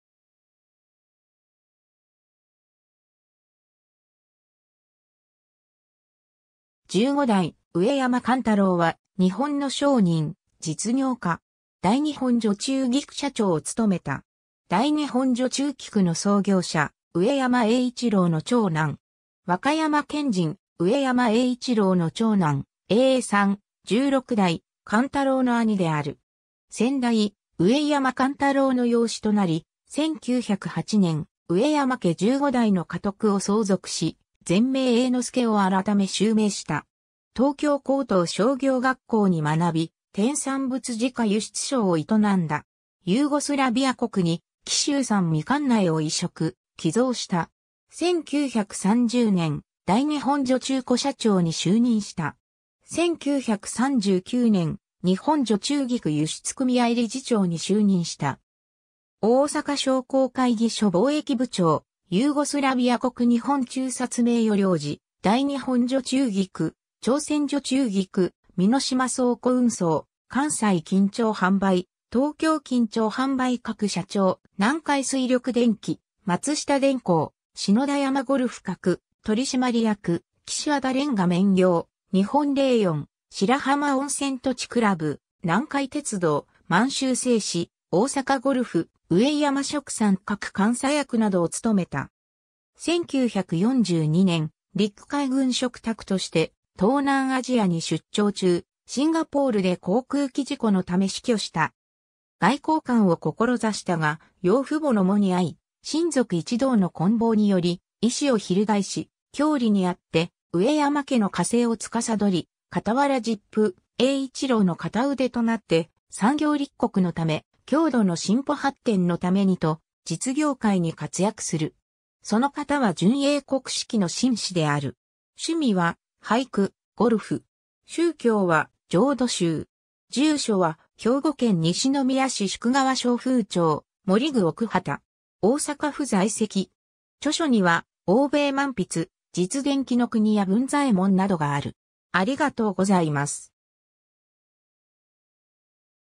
上山勘太郎 (15代)
15代上山 勘太郎（うえやま かんたろう、前名・英之助、1889年9月 - 1942年）は、日本の商人（天産物直輸出商）、実業家。
大日本除虫菊社長を務めた。
大日本除虫菊の創業者上山英一郎の長男。
 和歌山県人・上山英一郎の長男、英三、十六代勘太郎（前名・英夫）の兄である。
先代上山勘太郎の養子となり、1908年、上山家15代の家督を相続し、前名英之助を改め襲名した。
 東京高等商業学校に学び、天産物直輸出商を営んだ。
ユーゴスラビア国に紀州産蜜柑苗を移植、寄贈した。
1930年、大日本除虫粉社長に就任した。
1939年、日本除虫菊輸出組合理事長に就任した。
 大阪商工会議所貿易部長、ユーゴスラビア国日本駐箚名誉領事、大日本除虫菊、朝鮮除虫菊、箕島倉庫運送、関西金鳥販売、東京金鳥販売各社長、南海水力電気、松下電工、信太山ゴルフ各取締役、岸和田煉瓦綿業、日本レイヨン、白浜温泉土地倶楽部、南海鉄道、満州製紙、大阪ゴルフ、上山殖産各監査役などを務めた。
 1942年、陸海軍嘱託として東南アジアに出張中、シンガポールで航空機事故のため死去した。
 外交官を志したが、養父母の喪に遭い、親族一同の懇望により意志を翻し、郷里にあって上山家の家政を司り、傍ら実父英一郎の片腕となって産業立国のため、郷土の進歩発展のためにと、実業界に活躍する。
その型は純英国式の紳士である。
 趣味は俳句、ゴルフ。
宗教は浄土宗。
住所は兵庫県西宮市夙川松風町、森具奥畑。
大阪府在籍。
著書には『欧米漫筆』、『実伝紀伊国屋文左衛門』などがある。